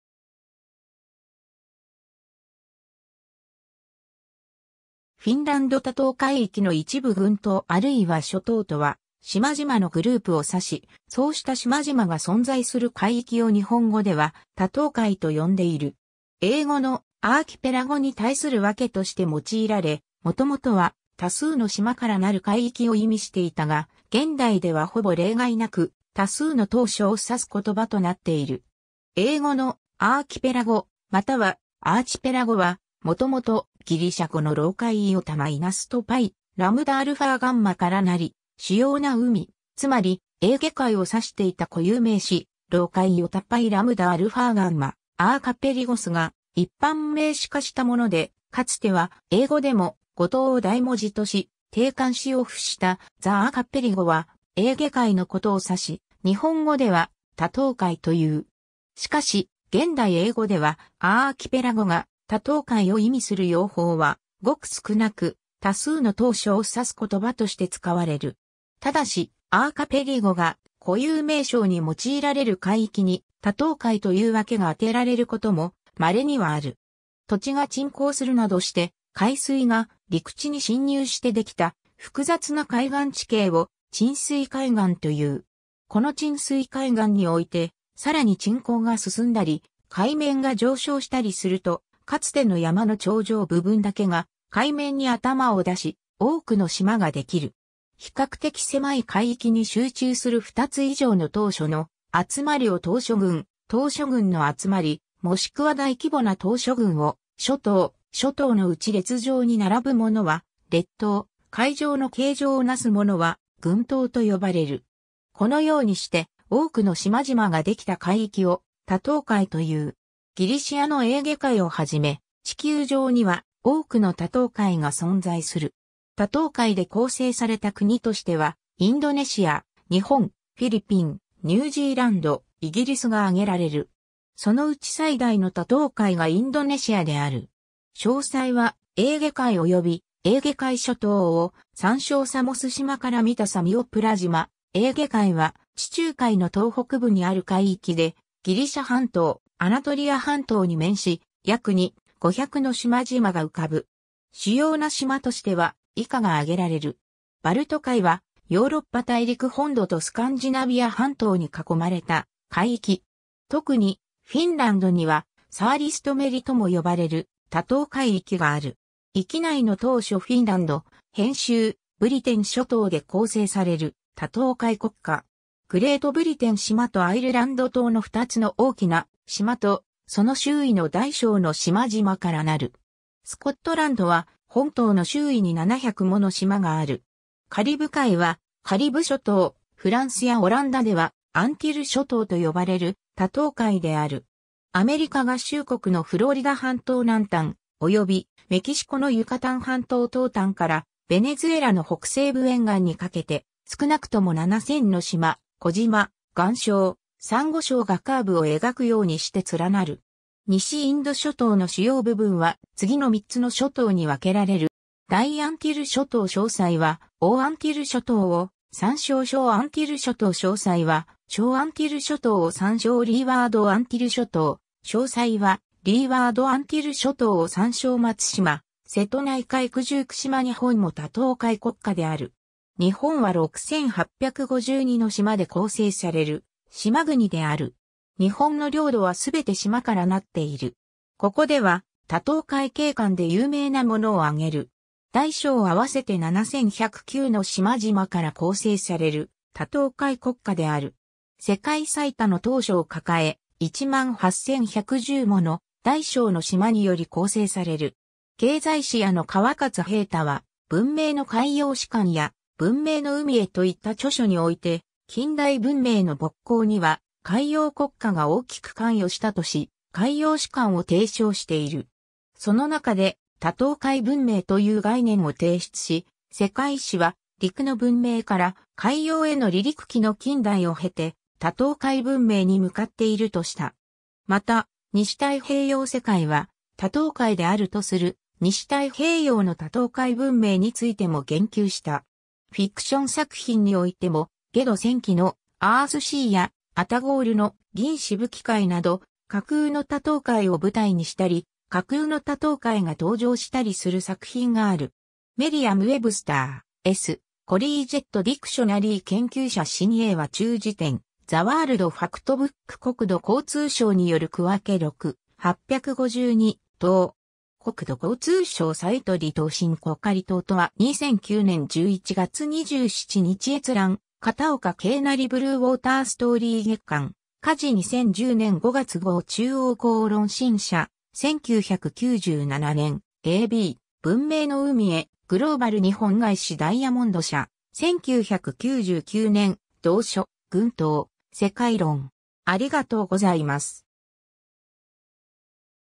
多島海
フィンランド多島海域の一部（レンピサーリ周辺） 群島（ぐんとう、英語: Archipelago）あるいは諸島（しょとう）とは島々のグループを指し、そうした島々が存在する海域を日本語では多島海（たとうかい）と呼んでいる。
 英語の「アーキペラゴ」（英: Archipelago）に対する訳として用いられ、もともとは「多数の島からなる海域」（多島海）を意味していたが、現代ではほぼ例外なく「多数の島嶼」（諸島・列島・群島）を指す言葉となっている。
 英語の「アーキペラゴ」（英: archipelago）または「アーチペラゴ」は、もともとギリシャ語の ἄρχι-（arkhi-、「主要な」）と πέλαγος（pelagos、「海」）からなり、「主要な海」、つまりエーゲ海を指していた固有名詞 Αρχιπέλαγος / Arkhipélagos が一般名詞化したもので、かつては英語でも語頭を大文字とし定冠詞を付した the Archipelago は、エーゲ海のことを指し、日本語では 「多島海」という。
しかし現代英語ではアーキペラゴが「多島海」を意味する用法はごく少なく、「多数の島嶼」（諸島・列島・群島）を指す言葉として使われる。
ただし、Archipelago が固有名称に用いられる海域（あるいは群島）に、「多島海」という訳が宛てられることも稀にはある。
 土地が沈降するなどして、海水が陸地に浸入してできた複雑な海岸地形を沈水海岸という。
この沈水海岸において、更に沈降が進んだり、海面が上昇したりすると、かつての山の頂上部分だけが海面に頭を出し、多くの島ができる。
比較的狭い海域に集中する2つ以上の島嶼の集まりを島嶼群、島嶼群の集まり、もしくは大規模な島嶼群を諸島、諸島のうち列状に並ぶものは列島、塊状の形状をなすものは群島と呼ばれる。
 このようにして多くの島々ができた海域を多島海という。
ギリシアのエーゲ海をはじめ、地球上には多くの多島海が存在する。
多島海で構成された国としては、インドネシア、日本、フィリピン、ニュージーランド、イギリスがあげられる。
そのうち最大の多島海がインドネシアである。
 詳細は「エーゲ海」および「エーゲ海諸島」を参照 サモス島から見たサミオプラ島 エーゲ海は、地中海の東北部にある海域で、ギリシャ半島、アナトリア半島に面し、約 2,500 の島々が浮かぶ。
主要な島としては以下が挙げられる。
 バルト海は、ヨーロッパ大陸本土とスカンジナビア半島に囲まれた海域。
 とくに、フィンランドにはサーリストメリとも呼ばれる多島海域（英: Archipelago Sea）がある。
 域内の島嶼 フィンランド[編集] ブリテン諸島で構成される多島海国家。
グレートブリテン島とアイルランド島の2つの大きな島と、その周囲の大小の島々から成る。
スコットランドは本島の周囲に700もの島がある。
 カリブ海は、カリブ諸島（西インド諸島）、フランスやオランダではアンティル諸島とよばれる多島海である。
アメリカ合衆国のフロリダ半島南端、および、メキシコのユカタン半島東端から、ベネズエラの北西部沿岸にかけて、少なくとも7000の島、小島、岩礁、珊瑚礁がカーブを描くようにして連なる。
 西インド諸島の主要部分は、次の3つの諸島に分けられる。
 大アンティル諸島 詳細は「大アンティル諸島」を参照 小アンティル諸島 詳細は「小アンティル諸島」を参照 リーワード・アンティル諸島 詳細は「リーワード・アンティル諸島」を参照 松島 瀬戸内海 九十九島 日本も多島海国家である。
日本は6,852の島で構成される島国である。
日本の領土はすべて島から成っている。
 ここでは多島海景観で有名なものを挙げる。
 大小合わせて7109の島々から構成される多島海国家である。
 世界最多の島嶼を抱え、1万8,110もの大小の島により構成される。
 経済史家の川勝平太（現静岡県知事）は、『文明の海洋史観』 や『文明の海へ』 といった著書において、近代文明の勃興には海洋国家（ヴェネツィア、ポルトガル、スペイン、オランダ、イギリス）が大きく関与したとし、「海洋史観」を提唱している。
そのなかで、「多島海文明」という概念を提出し、世界史は「陸の文明」から、「海洋への離陸期」の近代を経て、「多島海文明」に向かっているとした。
また「西太平洋世界は多島海である」とする「西太平洋の多島海文明」についても言及した。
 フィクション作品においても『ゲド戦記』の「アースシー」や『アタゴオル』の「銀しぶき海」など、...